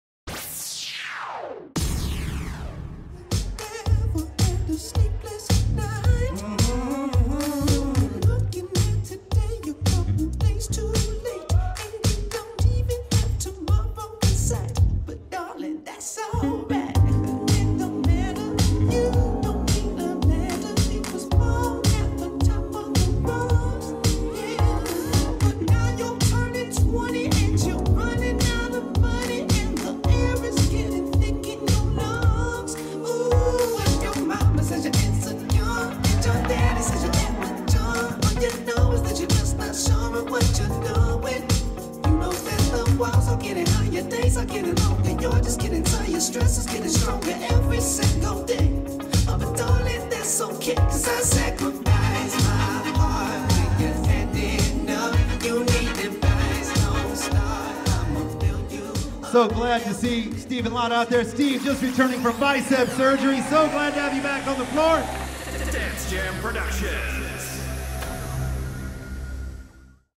DCSX 2022 Pro Staff Jam Steve Wilder & Lara Deni
Recorded at DCSX 2022. This annual event brings together nearly 1000 West Coast Swing dancers in the Washington, DC area for 4 days of non stop fun. Workshops, competitions and social dancing are enjoyed there. Video recording and editing by Moments of Dance LLC